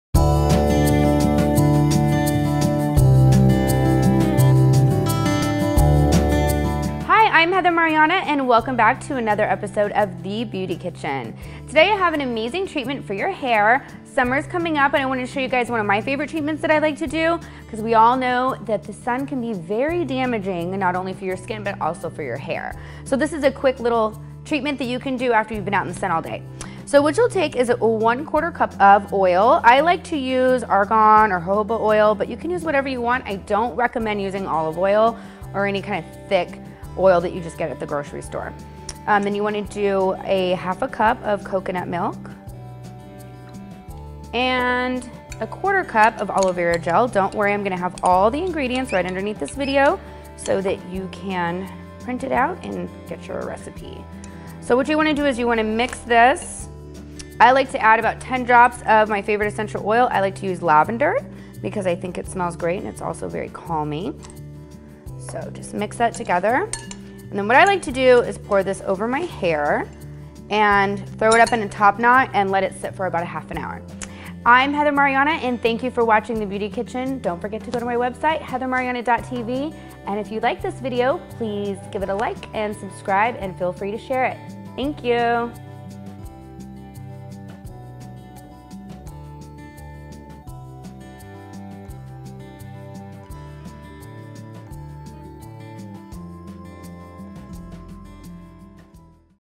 Summertime Hair Treatment
1/4 cup argon oil
1/2 cup coconut milk
1/4 cup aloe vera gel
10 drops of lavender
leave on for 30mins
rinse with warm water

heathermarianna.tv